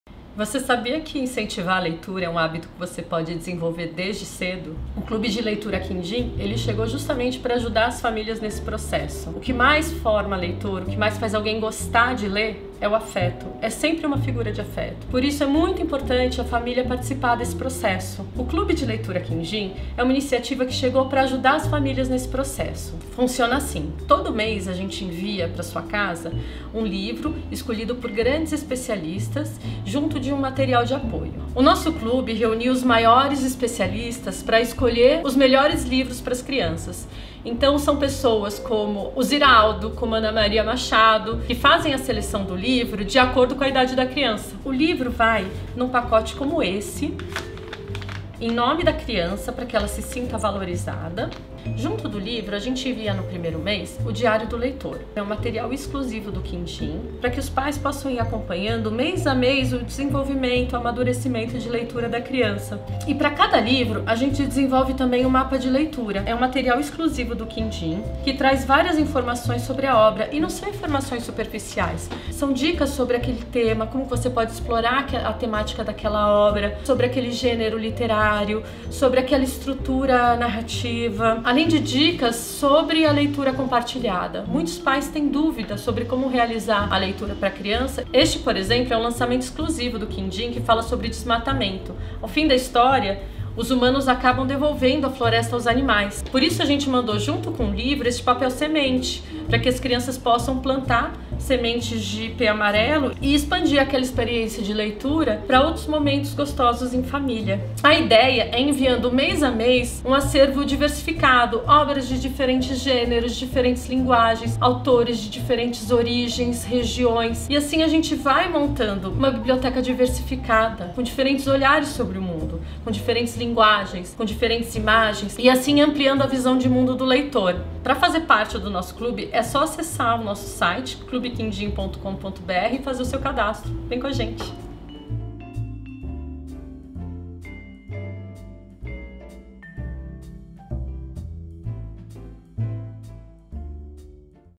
Conheça o Clube Quindim
Você já conhece o Clube Quindim? Um projeto independente de incentivo à leitura que busca formar leitores através do afeto.